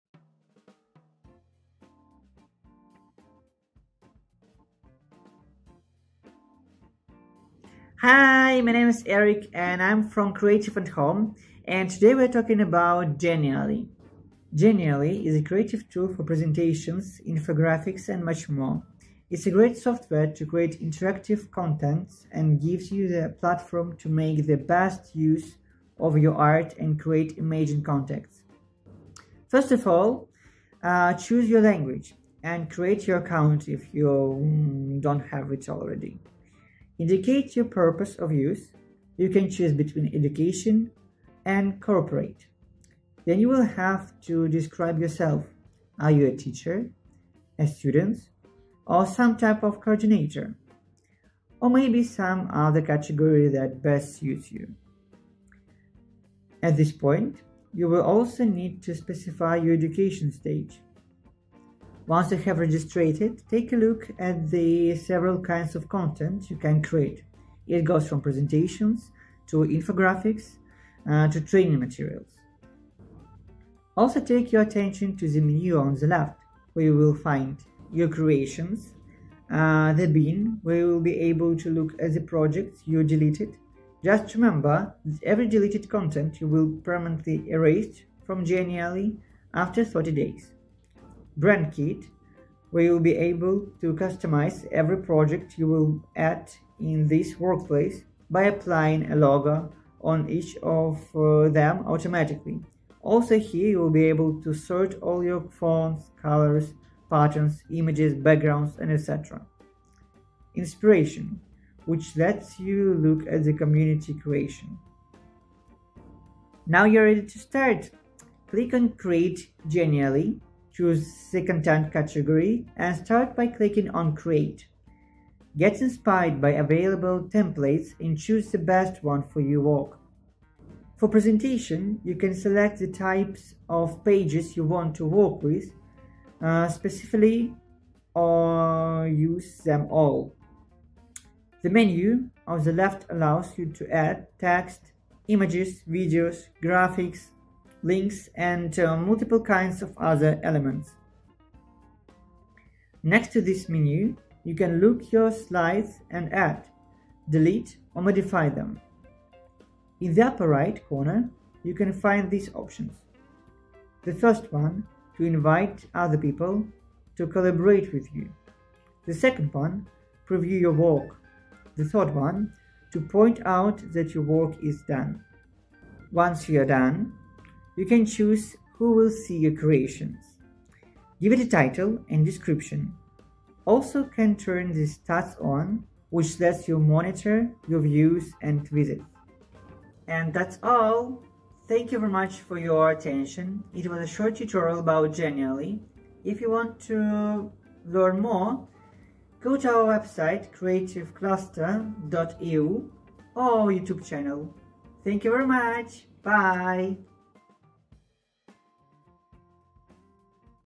Check our genially tutorial
The genial.ly application is a brilliant tool for creating interactive presentations and graphics that can be used, for example, in working with children and more. The basic functionalities of the program are available free of charge and you can use them to create interesting presentations, infographics and interactive boards.